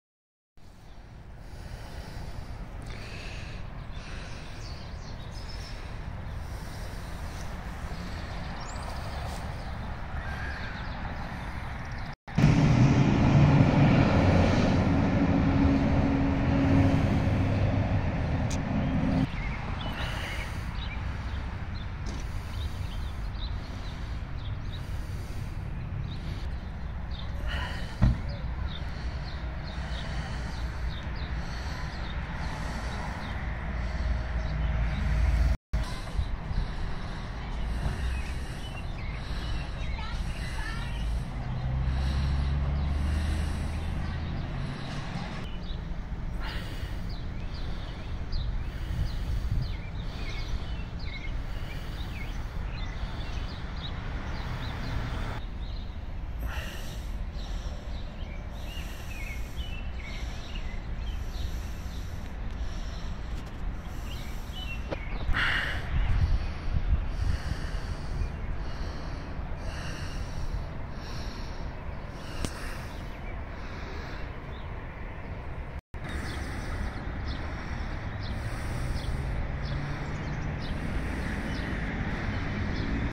Spring is a Time for Growth
Thought I'd give a glimpse of the wild plants that grow in our yard.